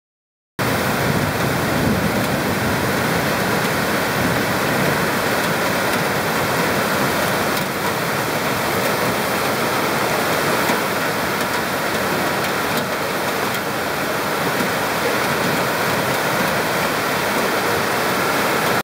🌧 Дождь Киев 11.07.2014
В Киеве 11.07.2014 прошел сильный дождь